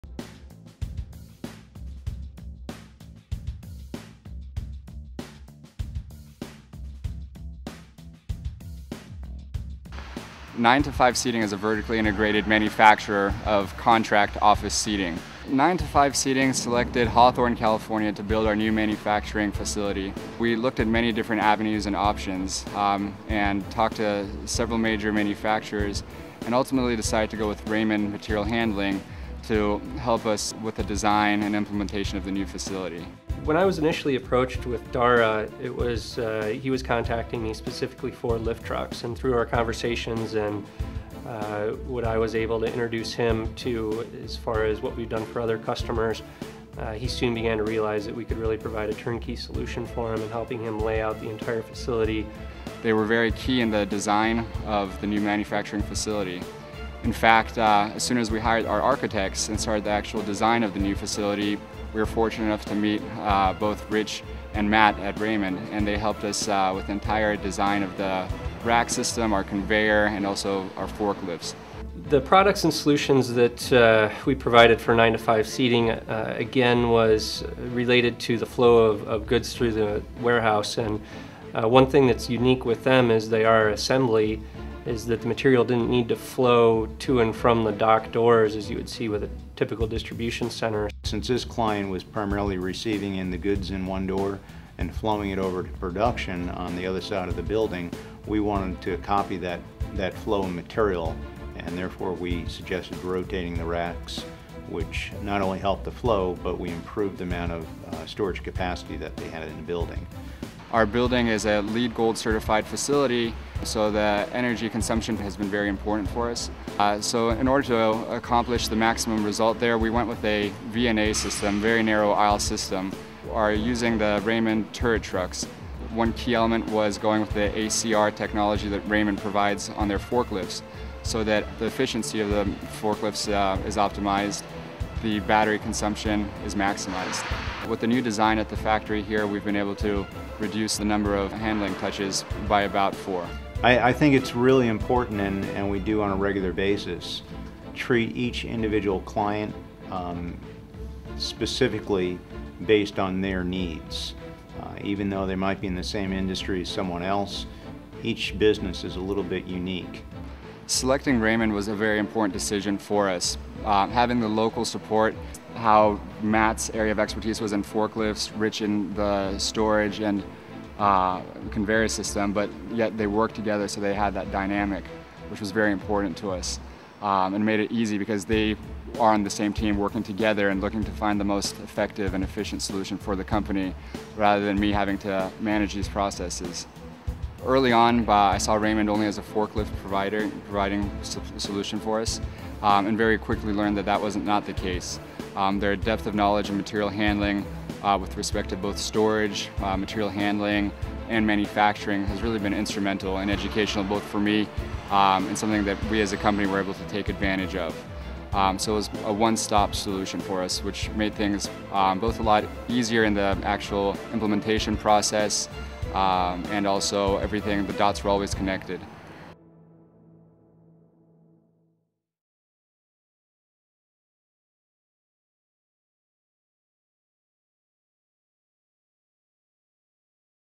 Warehouse Process Improvement | 9 to 5 Seating Case Study | Raymond Warehouse Optimization
9 to 5 Seating quadrupled production by implementing Raymond forklifts and conveyors to optimize the material handling process in a new LEED Gold-certified production and warehouse facility.